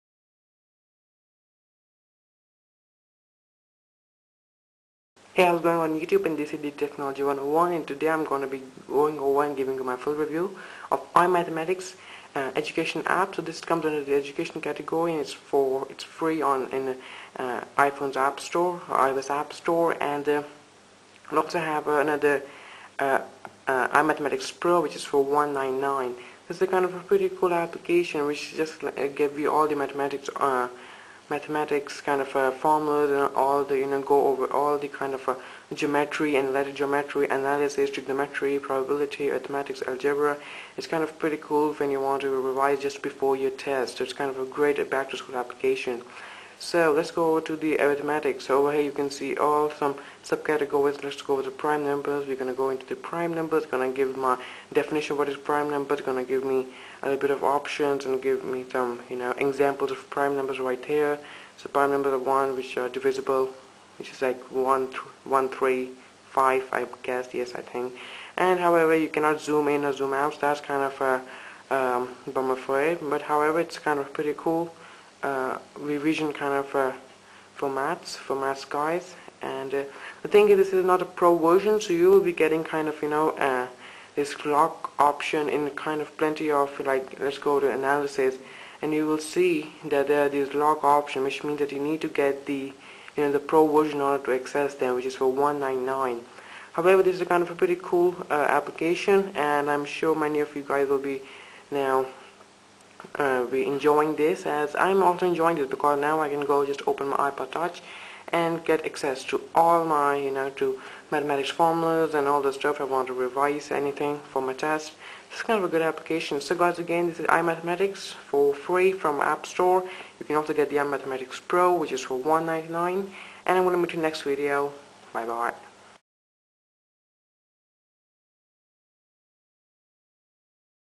App Review: iMathematics (Back to School)
For math nerds out there(including me) this is by far the best Back to School App. iMathematics.
Price: Free.